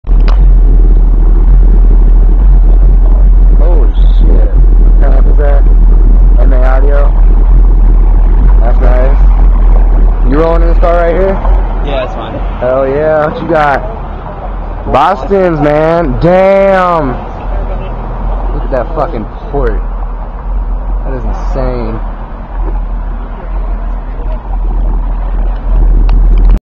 boston acoustics audio with Ma audio amp.
my friend owns the car, and started up his own team. hes got 4 boston single 2 ohm coil oval subs, and 1 Ma audio amp wired down to .5 ohms. system is so loud and powerful it will shut the car off. problem is that he can only play it for 5 seconds then it shuts down. i sat in it and about shit my pants.